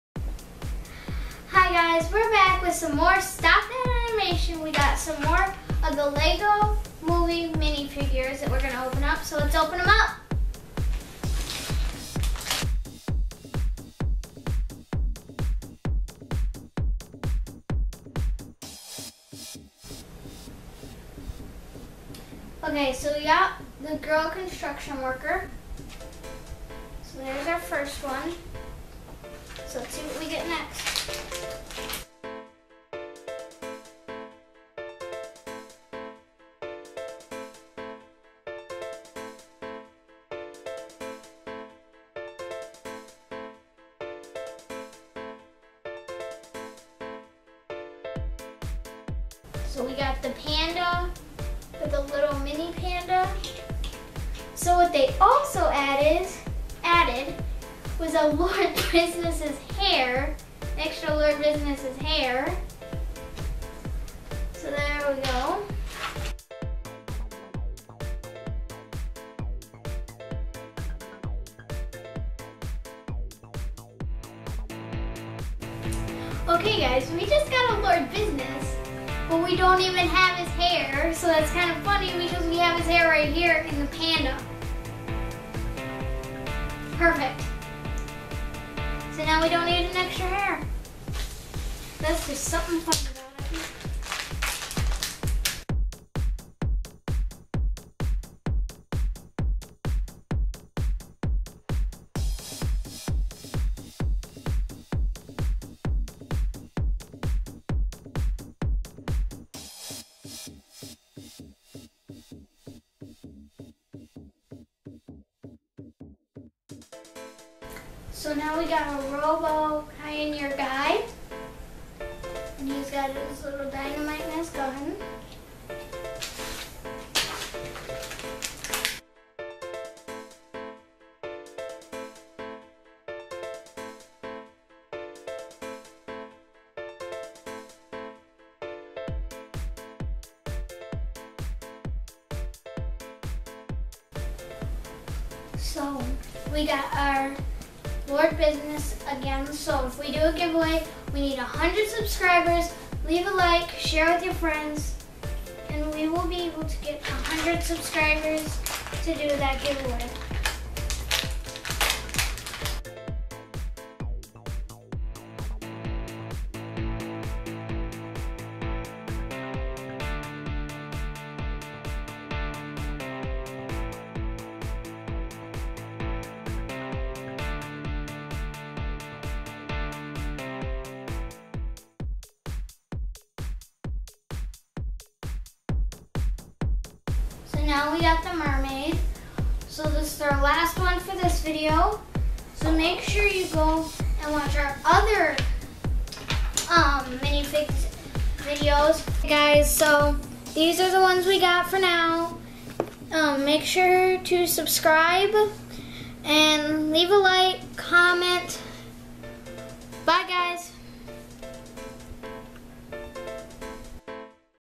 Lego Movie Minifigures Mystery Pack Opening
Watch as we open Lego Series 11 blind bag mystery pack mini figures.

Check out the Lego Simpsons Series 13 blind bag mystery pack mini figures videos here: